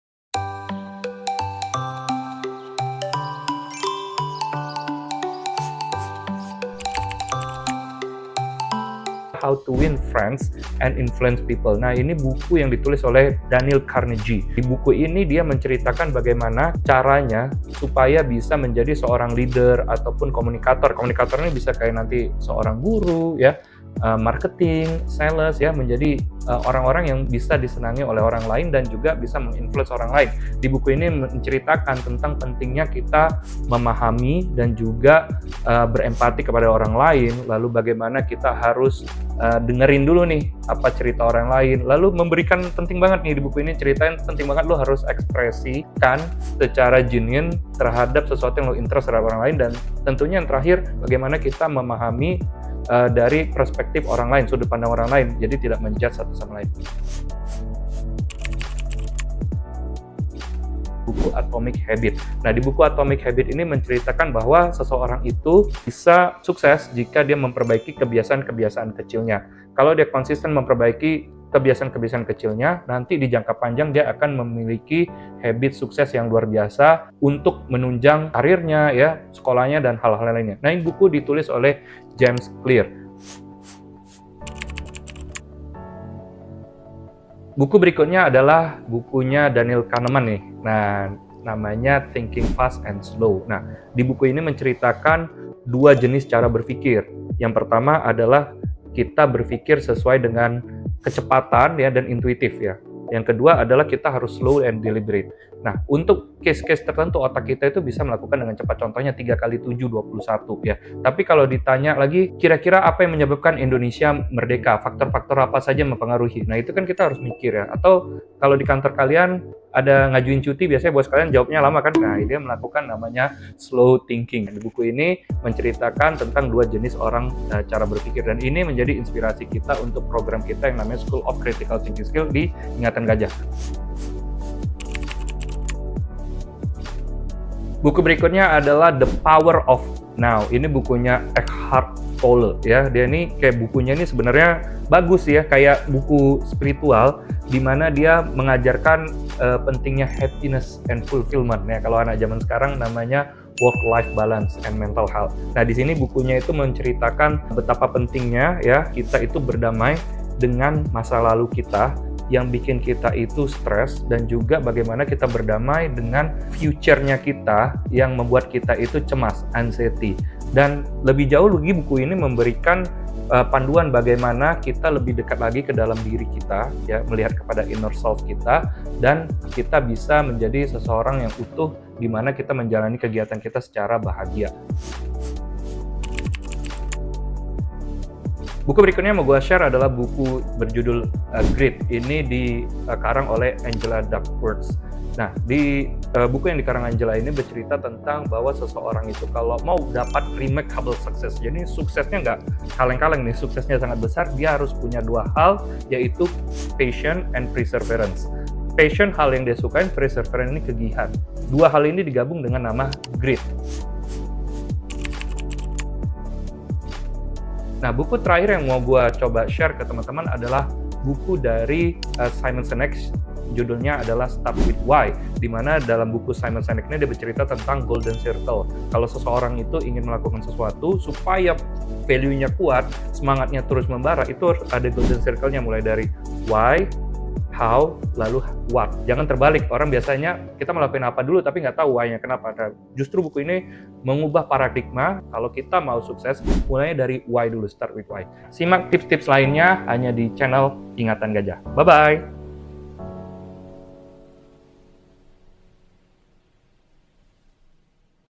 REKOMENDASI BUKU YANG BISA BUAT KAMU JADI PEMIMPIN YANG SUKSES
Meningkatkan keterampilan bersosialisasi, membangun hubungan yang tulus, dan memberikan pengaruh positif keapada orang lain merupakan 3 hal yang menurut Dale Carnegie penting dimiliki seorang leader. Dale Carnagie merupakan penulis buku "How to Win Friends & Influence People", buku ini bisa membuka pikirannya untuk menjadi seorang leader dan communicator yang sukses.

Di sisi lain, kesuksesan juga bisa kamu bangun dari hal-hal kecil. Menurut James Clear, dalam bukunya yang berjudul Atomic Habit, kita bisa sukses dengan mulai memperbaiki kebiasaan kecil kita secara konsisten.

Banyak cara yang bisa kalian lakukan untuk mencapai kesuksesan, kalian bisa belajar dari berbagai tokoh hebat melalui buku-buku yang mereka tulis. Selain, dua buku di atas, IngatanGajah sudah merangkum 4 buku lainnya yang cocok untukmu. Simak videonya ya!